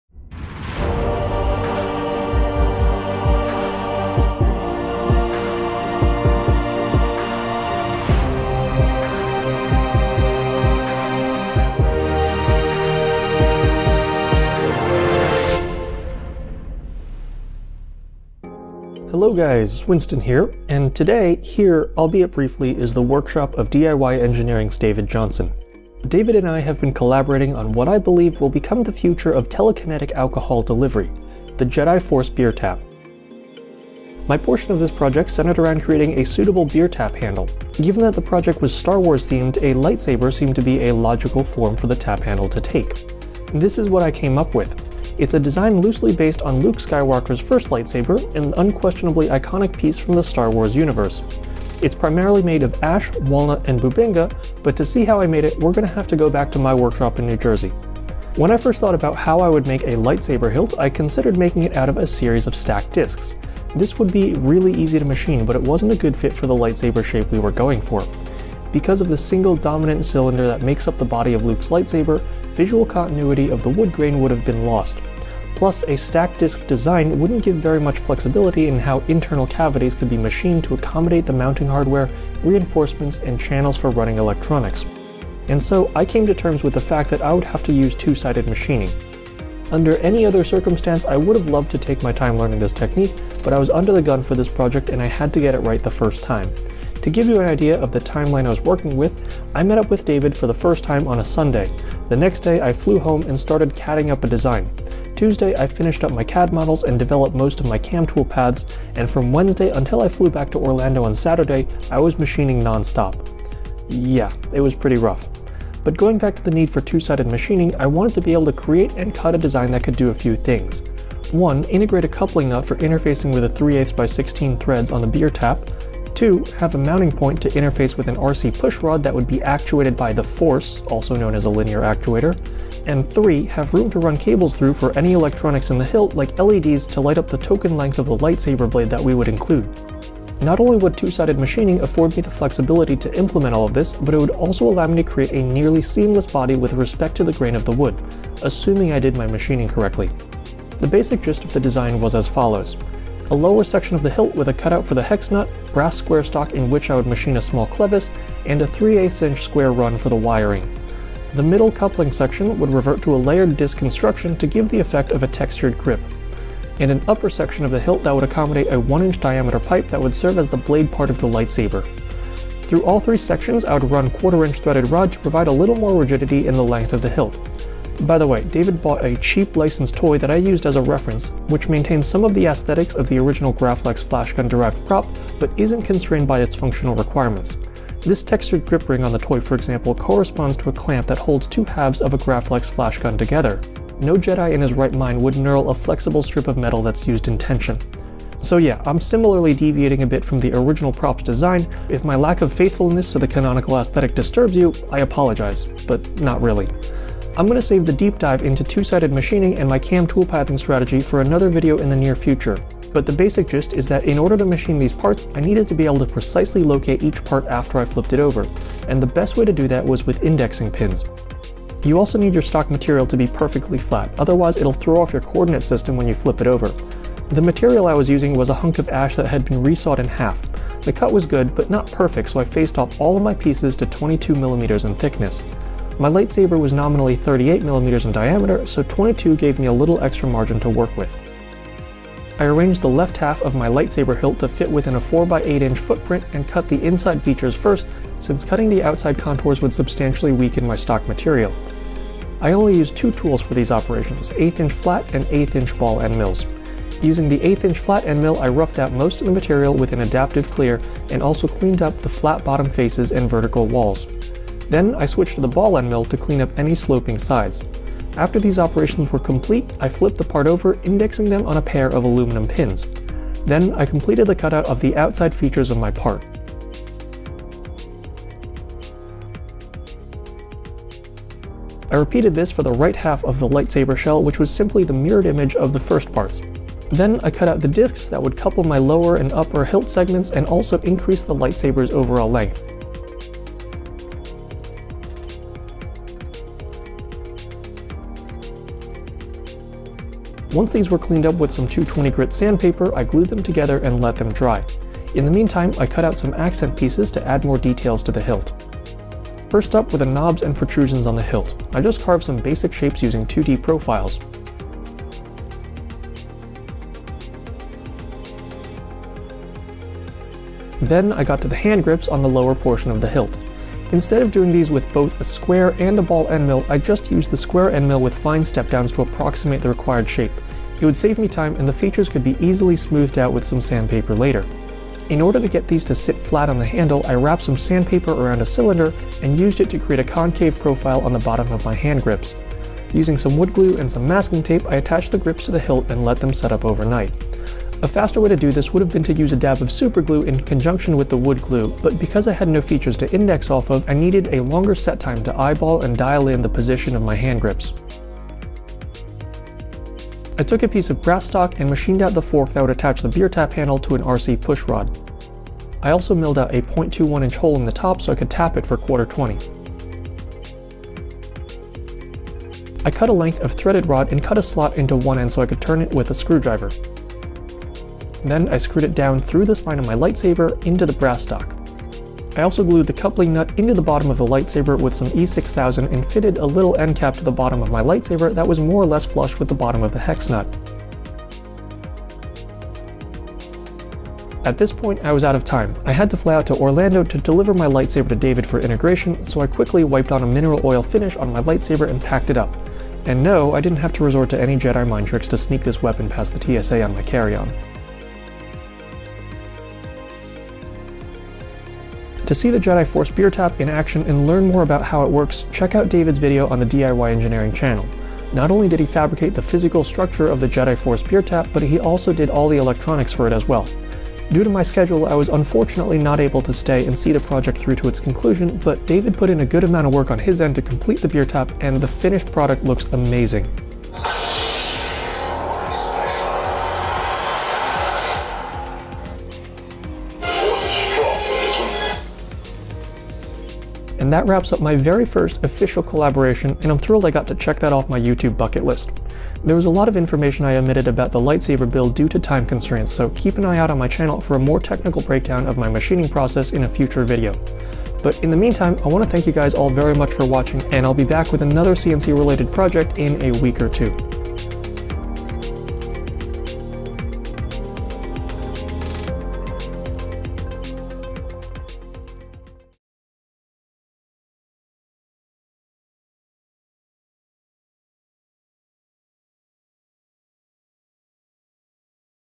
Wooden Lightsaber Handle for the Jedi Force Beer Tap - CNC Project #108
I collaborated with David Johnson of DIY Engineering to create the ultimate Star Wars beer tap. I machined a lightsaber beer tap handle, while he tackled the electronics and frame.